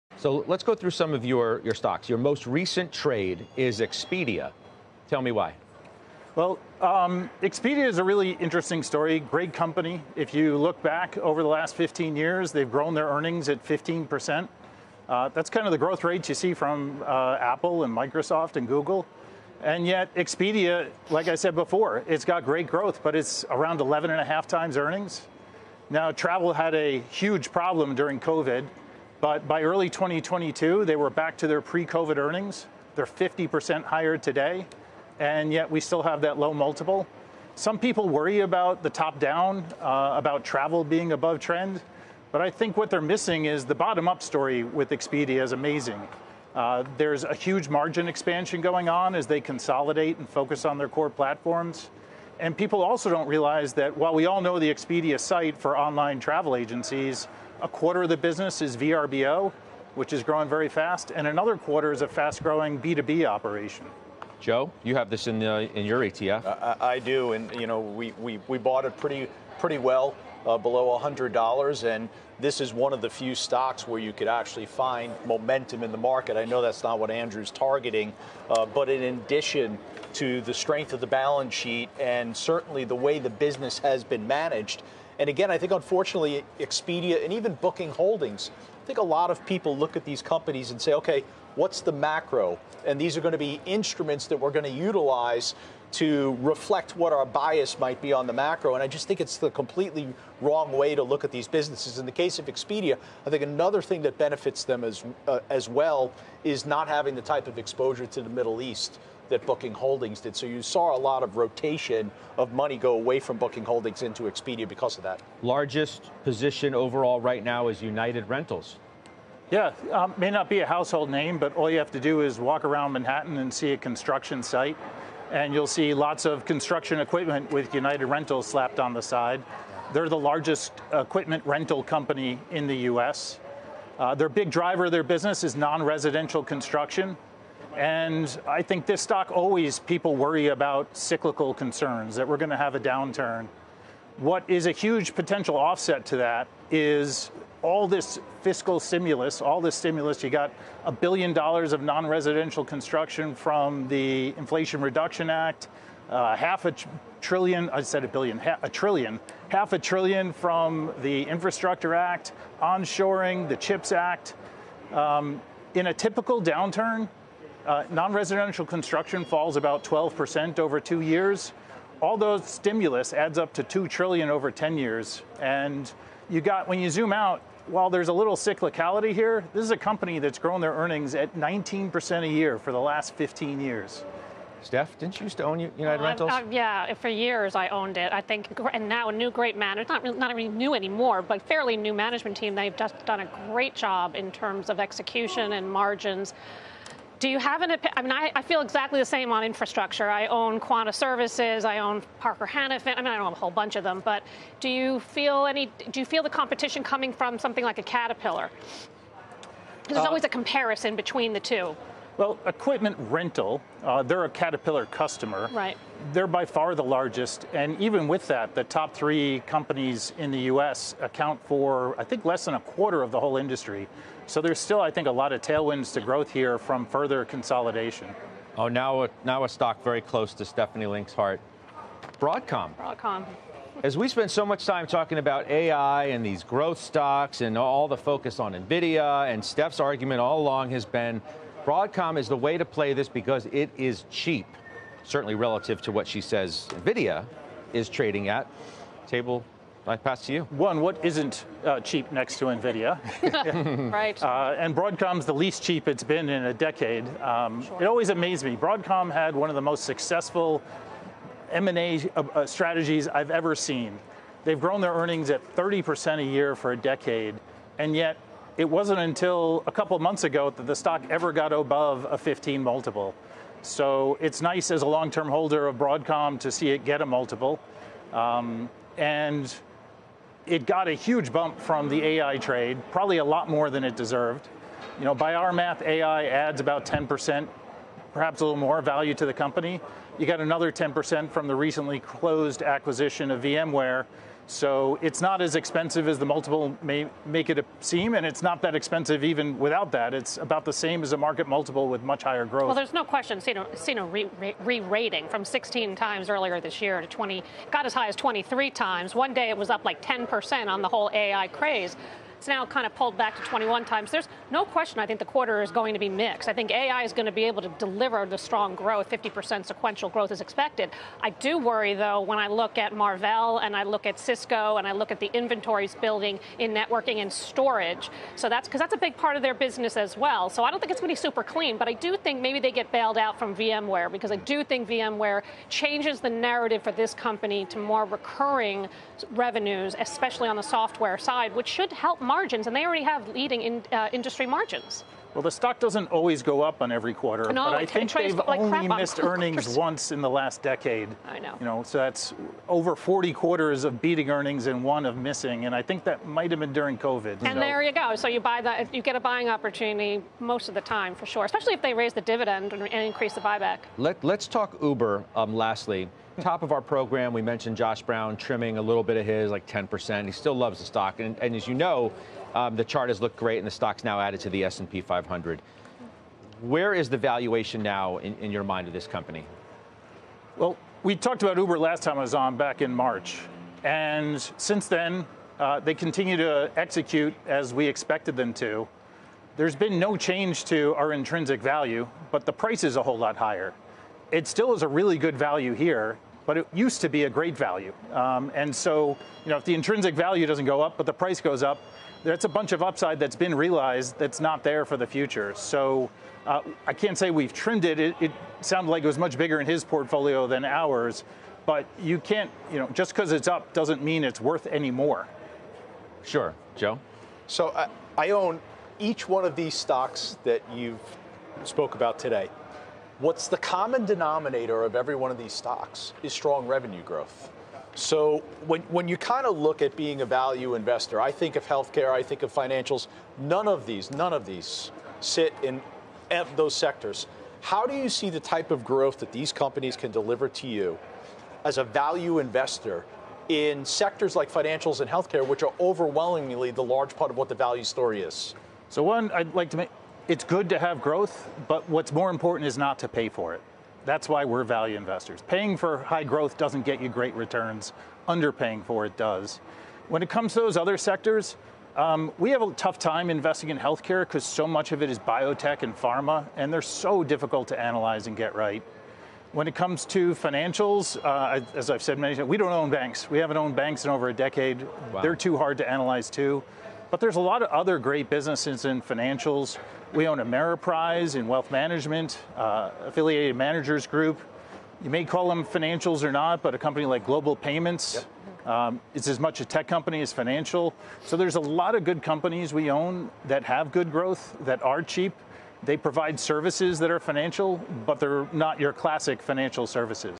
Top value investor Andrew Wellington breaks down his top stock picks
Andrew Wellington, Lyrical Asset Management Co-Founder & CIO, discusses his top value holdings.